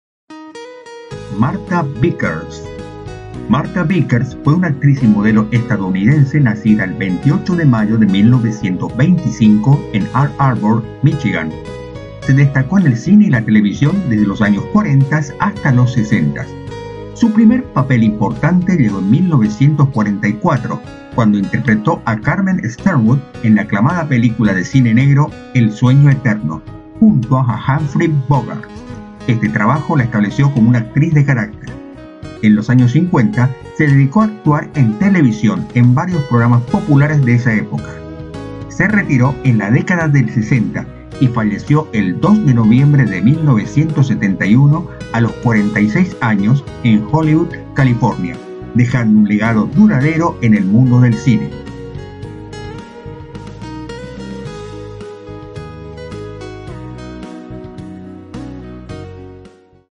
Martha Vickers, actriz. Nace 28 de Mayo de 1925
28 de Mayo de 1925, nace la actriz y modelo estadounidense MARTHA VICKERS
May 28th, 1925; american model and actress MARTHA VICKERS was born